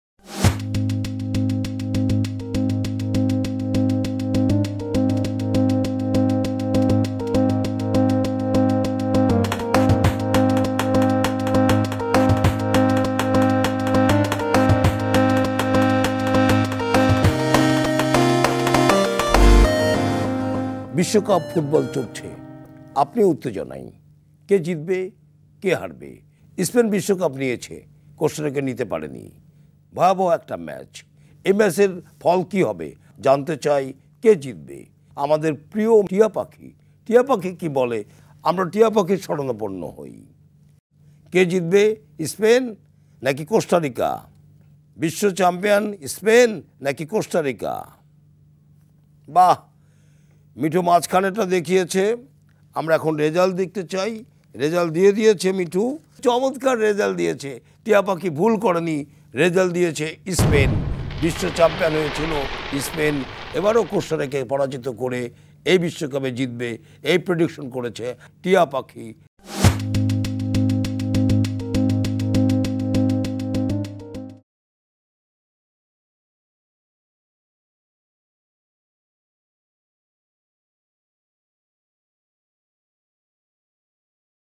স্পেন-কোস্টারিকা ম্যাচের ফলাফল নির্ধারণ করলো টিয়া পাখি! || Spain vs Costa Rica || Ekushey TV
স্পেন-কোস্টারিকা ম্যাচের ফলাফল নির্ধারণ করলো টিয়া পাখি!
স্পেন-কোস্টারিকাম্যাচে জিতবে কোন দল?